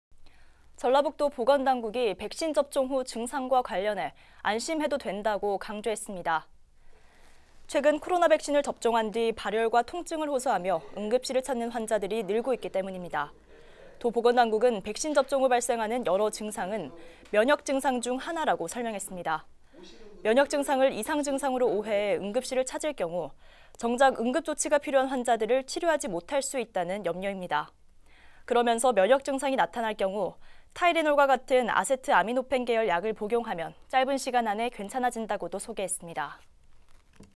코로나 백신 접종 뒤 발열‥응급실 이용 자제 당부
전라북도 보건당국이
백신 접종 후 증상과 관련해
안심해도 된다고 강조했습니다.

최근 코로나 백신을 접종한 뒤
발열과 통증을 호소하며
응급실을 찾는 환자들이 늘고 있기 때문입니다.